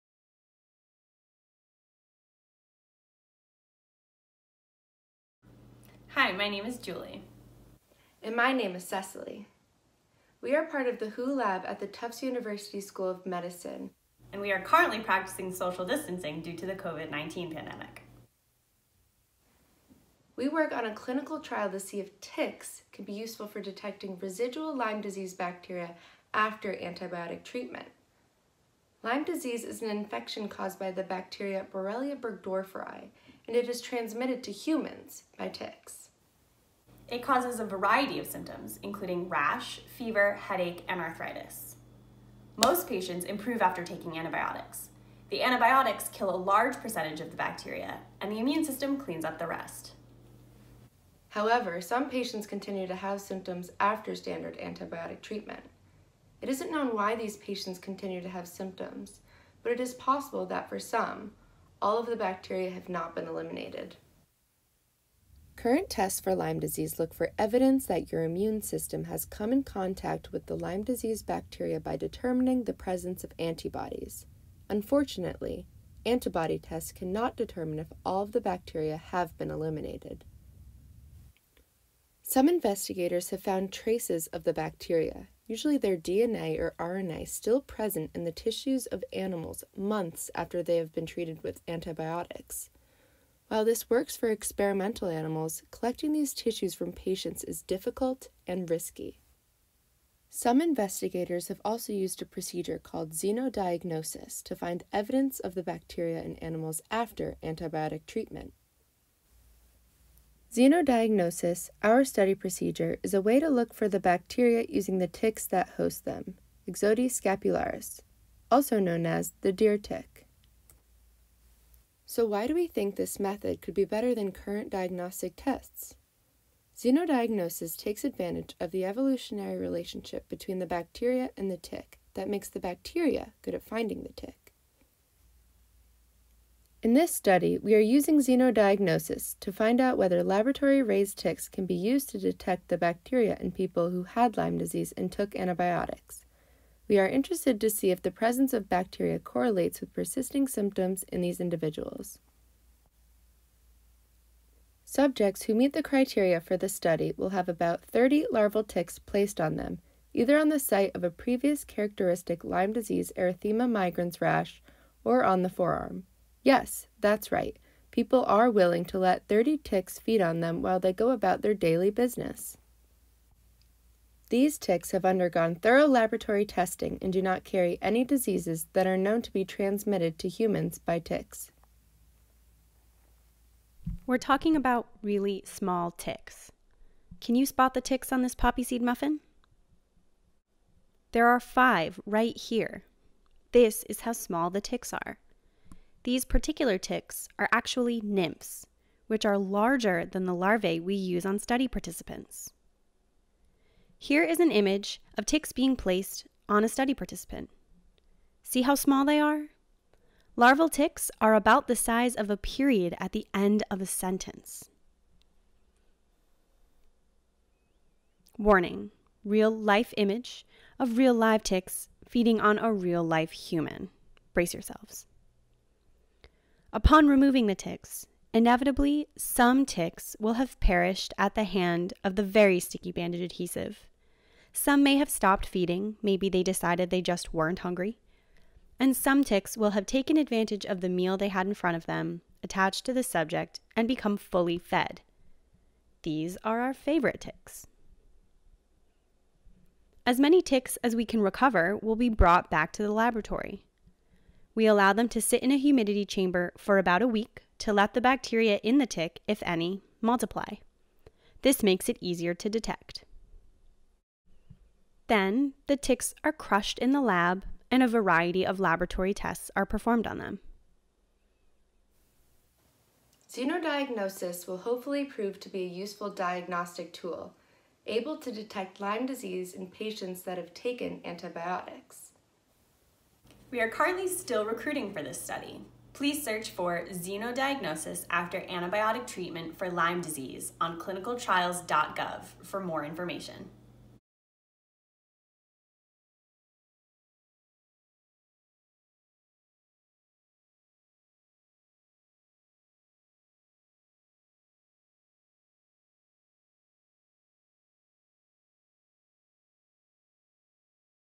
Feeding Ticks on People: how to use ticks as a diagnostic tool
The persistence of bacteria or its components after antibiotic therapy is controversial.  One method for detecting the presence of the organism is to use its natural vector, the Ixodes tick, to draw the bacteria out of tissues and into the tick.  The tick can then be tested for the presence of the bacteria.  This is called Xenodiagnosis.

Watch our study coordinators, Julie McCarthy and Cecily Freliech, describe xenodiagnosis and our study in patients with Lyme disease.